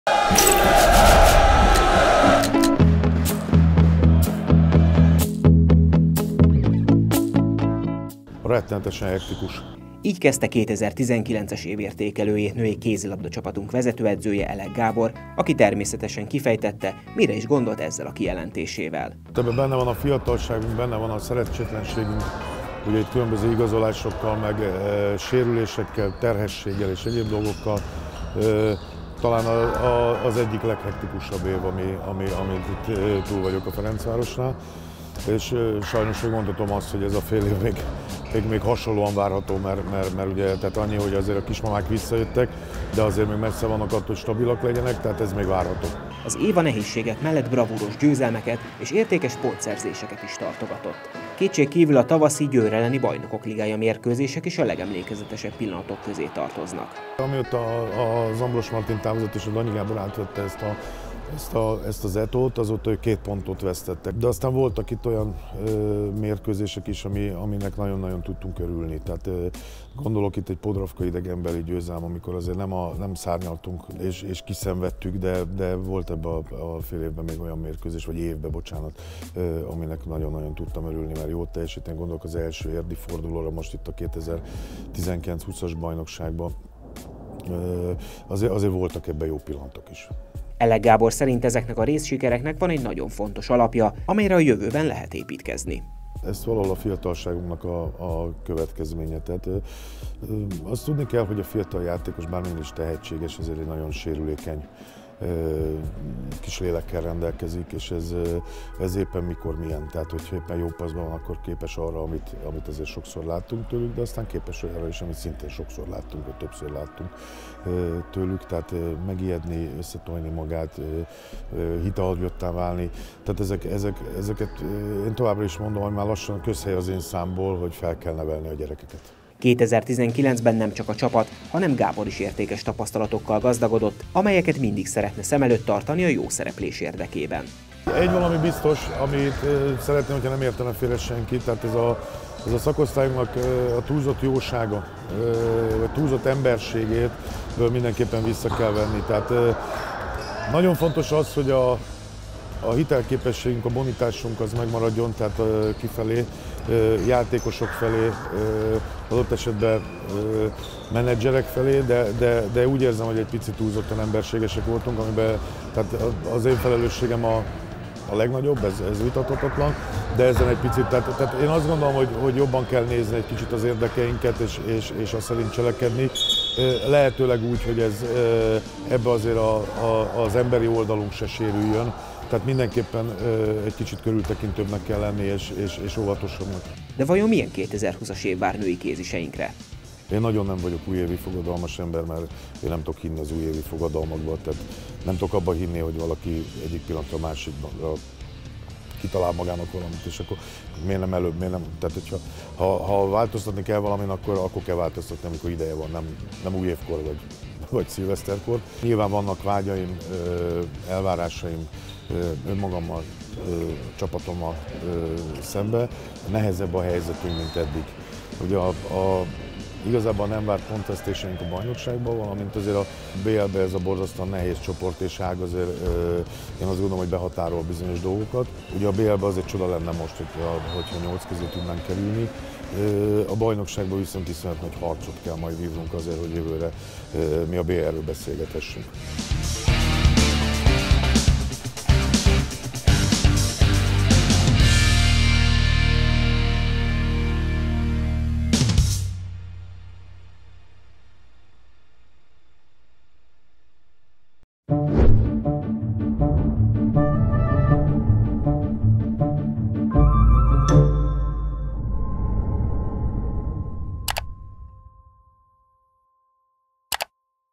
FM | Elek Gábor őszinte véleménye a 2019-es évről | 2020. 01. 15.
Nehézségek, bravúros győzelmek és pontszerzések. Röviden így lehetne összefoglalni női kézilabdacsapatunk tavalyi évét. Vezetőedzőnkkel, Elek Gáborral most sorra vettük az FTC-Rail Cargo Hungaria 2019-es esztendőjének meghatározó, olykor rendkívül emlékezetes, máskor pedig borzasztóan nehéz pillanatait.